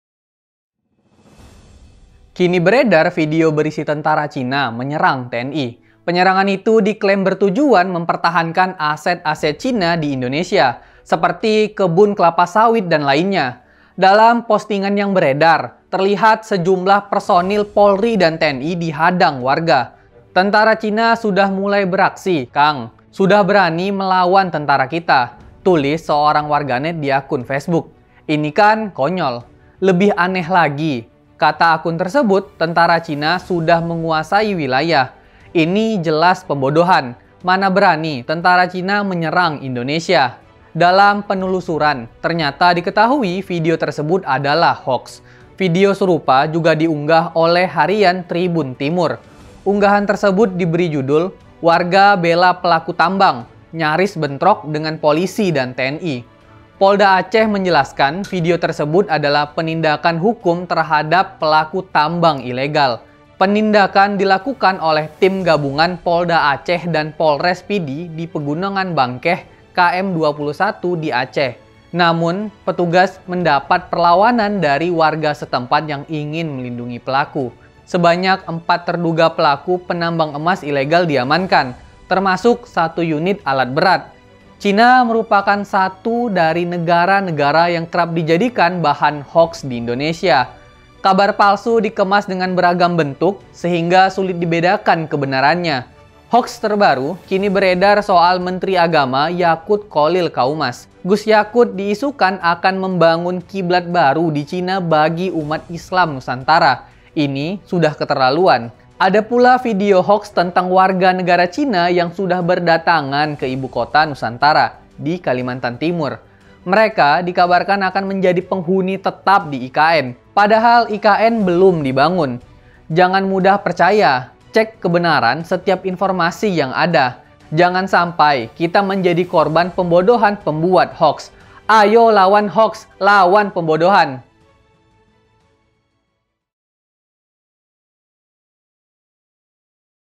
HOAKS! TENTARA CHINA SERANG TNI | Belmondo Scorpio
Aduh, perasaan berita hoax nggak ada habisnya ya! Contohnya aja berita hoax yang bilang tentara China menyerang TNI. Malah katanya tentara Cina sudah menguasai wilayah di Indonesia. Itu HOAX ya guys! Buat kalian, hati-hati deh sama berita hoax. Cek dulu kebenaran tiap berita yang kita terima. Jangan sampe kita dibodohi berita hoaks!